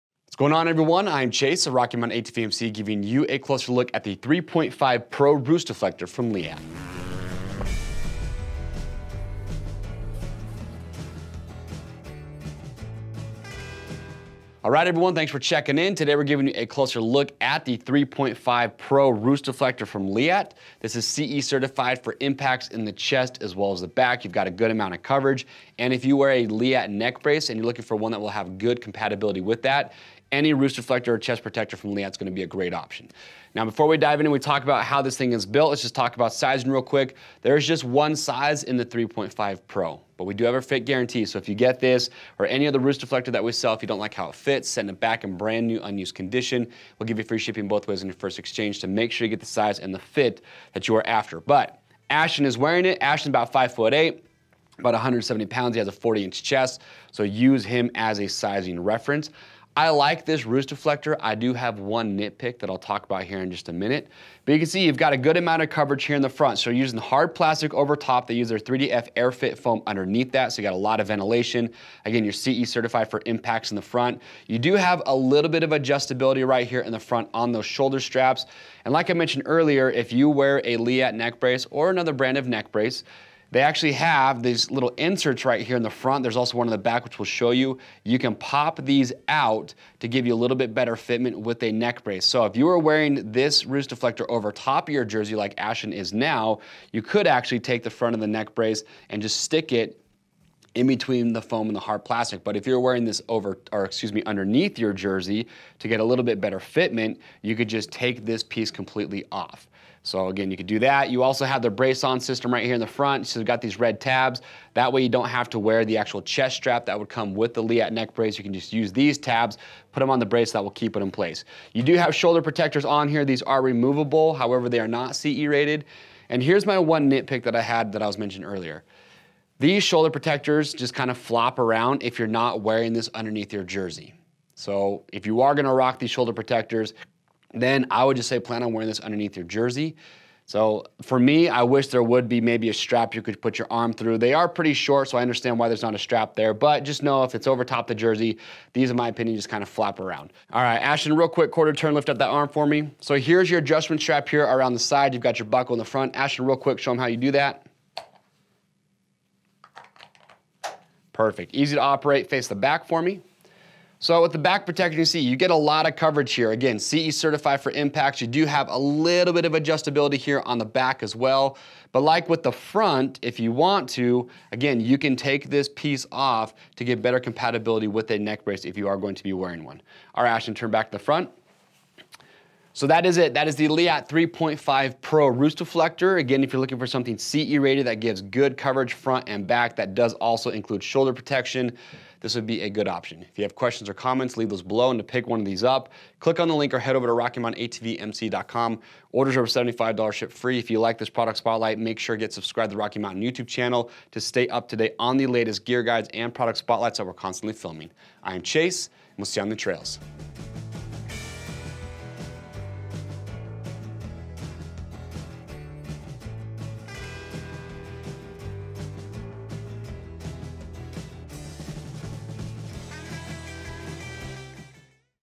Leatt 3.5 Pro Motocross Roost Deflector
The Leatt 3.5 Pro Roost Deflector features a lightweight hard shell with 3DF AirFit ventilated soft impact foam with CE level 1 chest and back protection as well as 3DF impact foam in the shoulder guards. Removable front and rear plates and BraceOn neck brace fitting system allow for an over or under the shirt fitment with your favorite Leatt motocross neck brace.